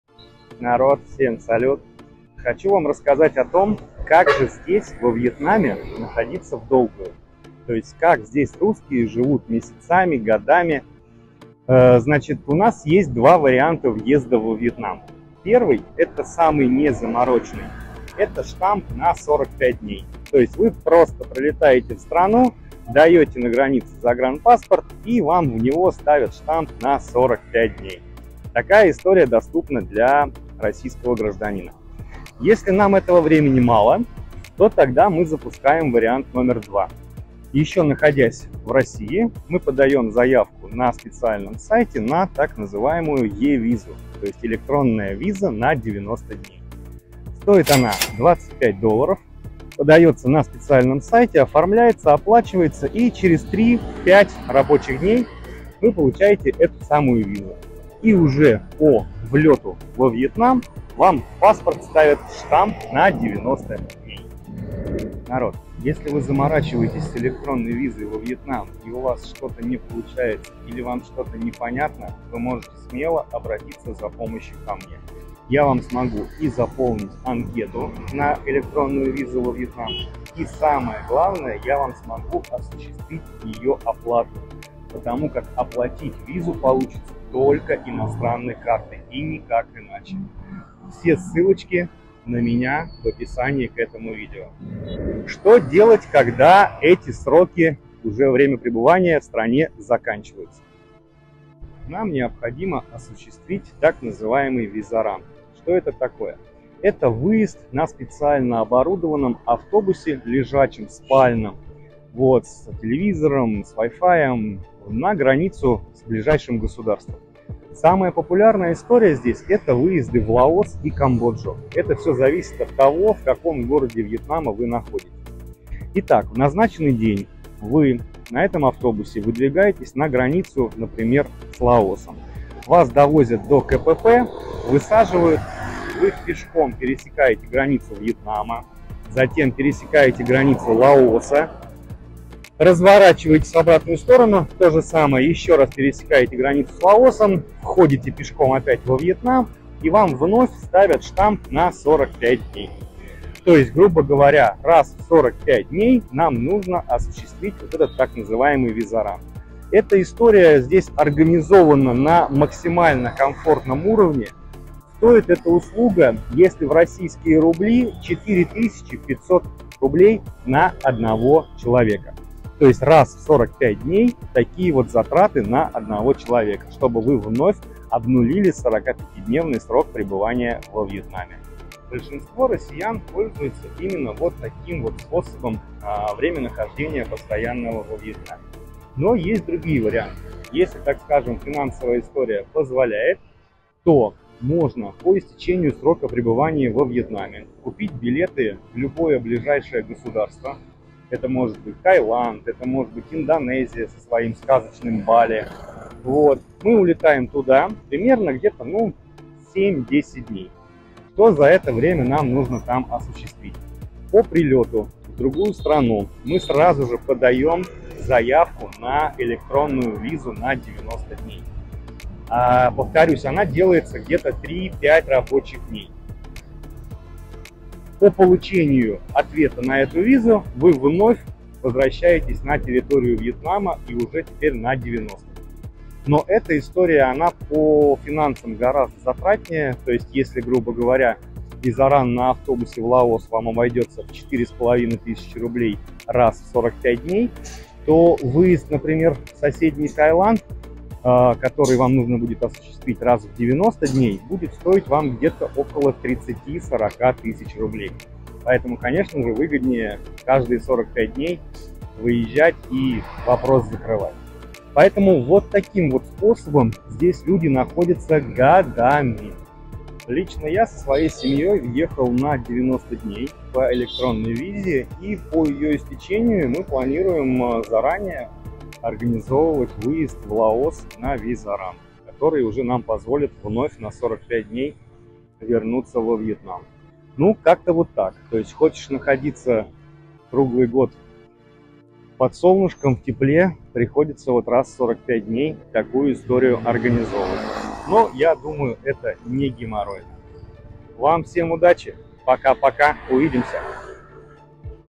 Как жить во Вьетнаме на постоянной основе, виза во Вьетнам
Рассказываю как люди находятся и живут на постоянной основе во Вьетнаме месяцами и годами. Что такое визаран и с чем его едят.